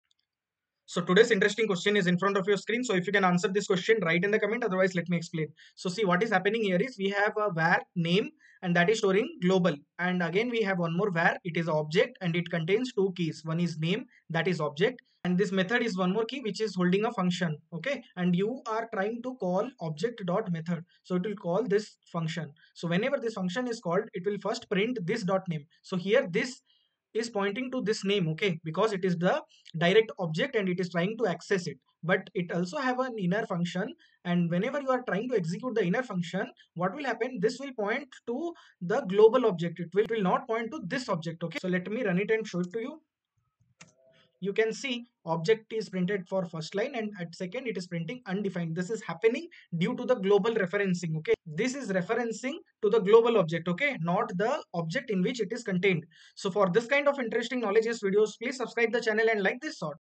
Javascript Interview Question asked in FAANG
In this short video, we cover some of the most commonly asked JavaScript output-based interview questions that top MNCs and FAANG companies ask during technical rounds. These questions test your understanding of JavaScript concepts like hoisting, closures, async behavior, and more.

If you're preparing for frontend interviews or web developer roles, these JS interview questions will help boost your confidence and improve your chances of success.

Watch till the end to challenge your JavaScript knowledge and comment your score!